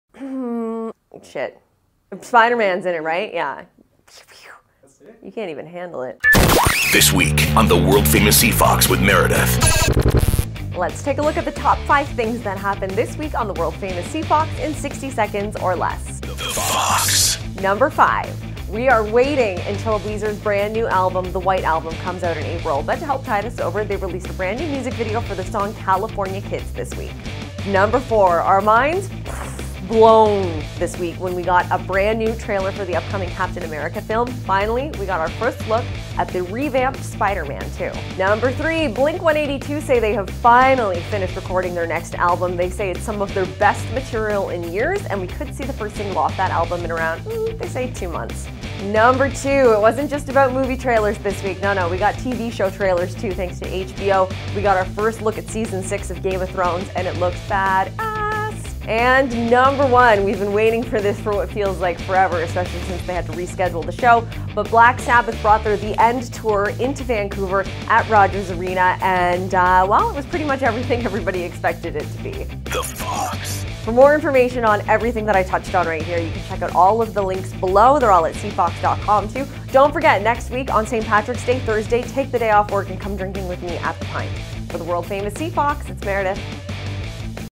This Week On CFOX March 11 2016
Take a look at the top 5 things that happened on The Fox this week with Meredith.